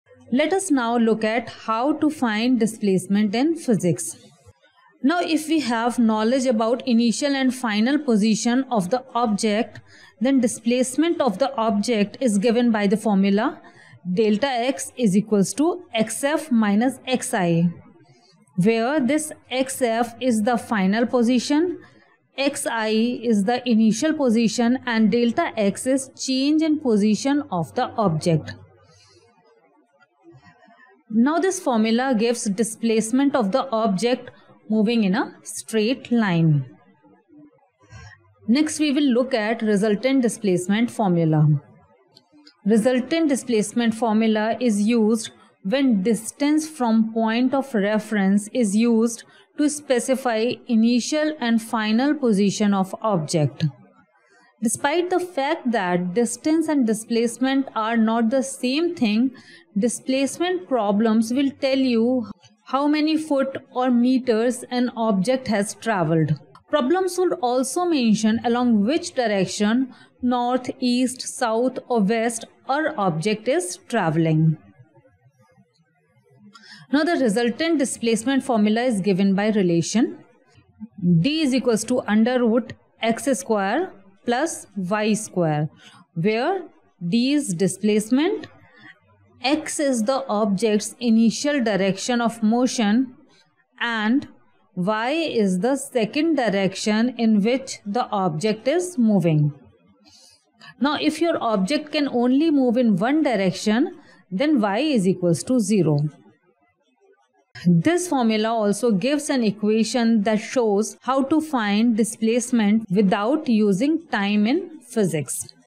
Displacement Formula in Physics
Learn about the formula for displacement in physics.
If we have knowledge about the initial and final position of the object then displacement of the object is given by the formula
Δ x=x_f-x_i

where,
x_f is the final position
x_i is the initial position
Δ x is a change in the position of the object

Now this formula gives the displacement of the object moving in a straight line.

## resultant displacement formula
The resultant displacement formula is used when the distance from point of reference is used to specify the initial and final position of the object. Despite the fact that distance and displacement are not the same thing, displacement problems will tell you how many "foot" or "meters" an object has traveled. Problems would also mention along which direction (North, east, south, west) our object is traveling.

The resultant displacement formula is given by relation
d=√{x^2+y^2}
where, $d$ is displacement.
x is the object's initial direction of motion and
y is the second direction in which the object is moving.
If your object can only move in one direction, $y = 0$.$.
This is the formula for resultant displacement. This formula also gives an equation that shows how to find displacement without using time in physics.